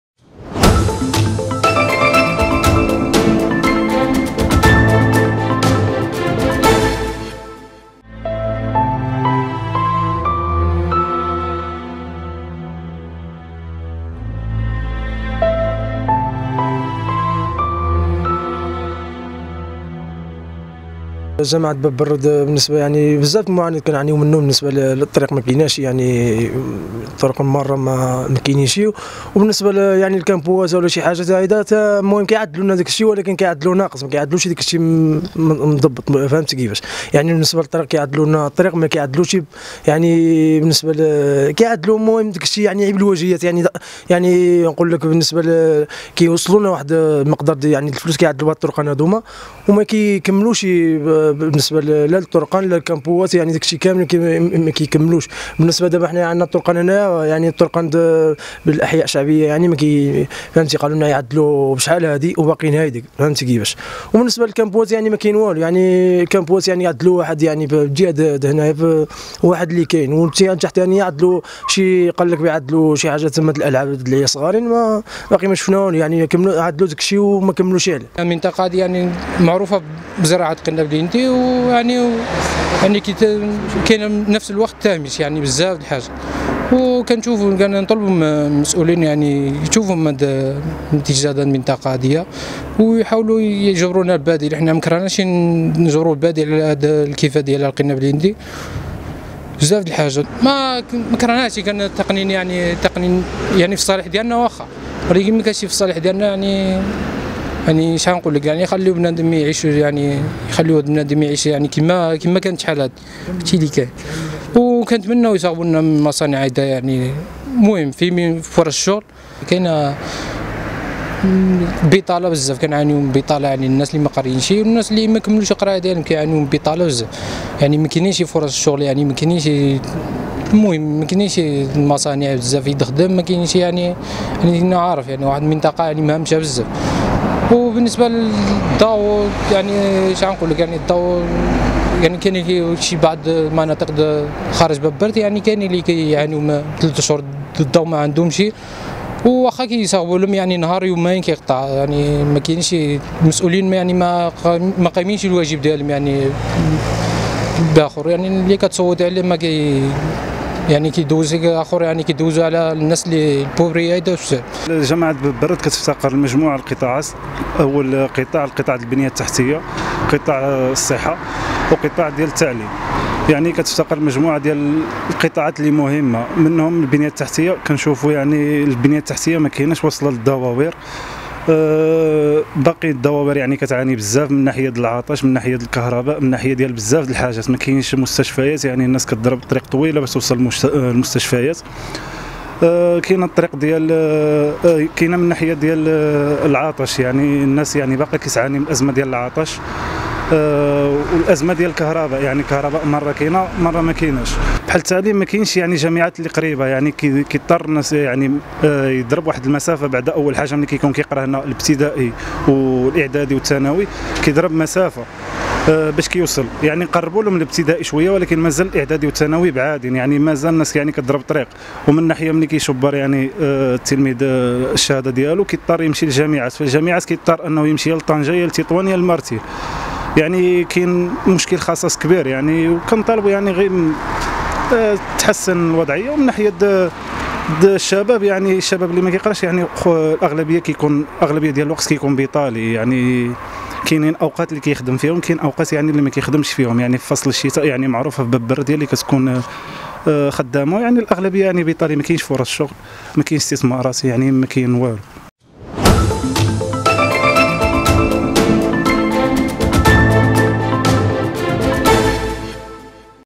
جماعة باب برد بشفشاون.. تهميش وبطالة وفقر وطرقات لا تمشي عليها حتى الدواب
جماعة باب برد.. تهميش وبطالة وفقر وطرقات لا تمشي عليها حتى الدواب

# أخبار_عاجلة